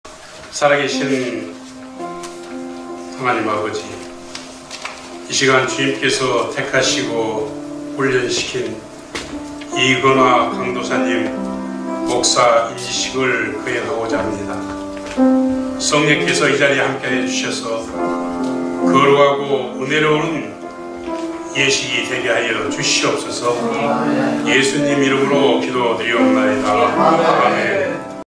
기원 - 노회장 양경욱 목사
이건화 강도사 목사 안수식
주관 : 대한예수교장로회(합동)글로벌총회 미동부노회(노회장 : 양경욱 목사)
2022년 4월 12일(화) 오전 11시 30분 @주예수사랑교회